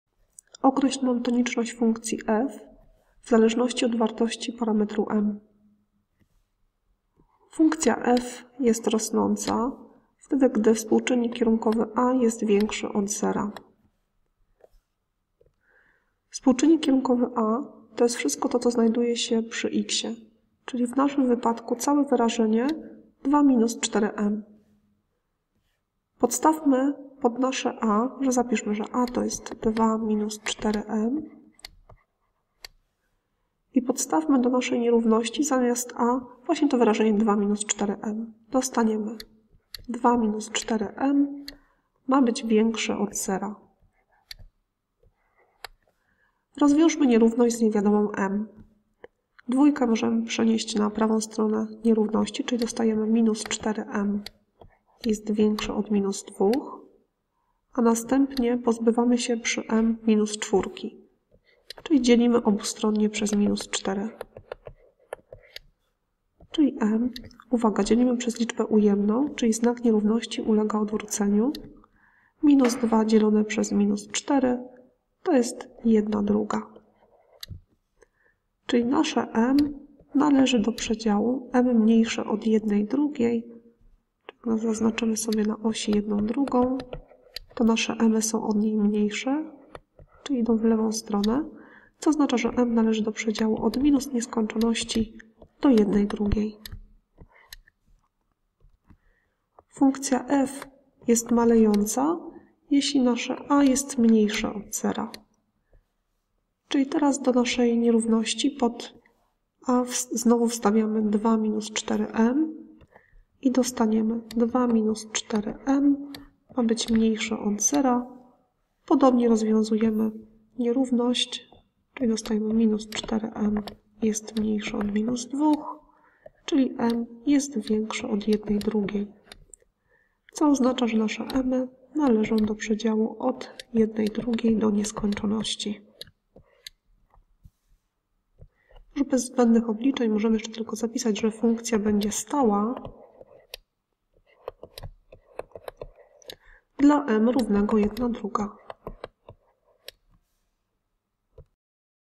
Monotoniczność funkcji liniowej
Określ monotoniczność funkcji w zależności od parametru m.

Film powstał jako pomoc do kart pracy, które znajdują się na stronie w zakładce: MATMA: FUNKCJA LINIOWA: WŁASNOŚCI FUNKCJI LINIOWYCH.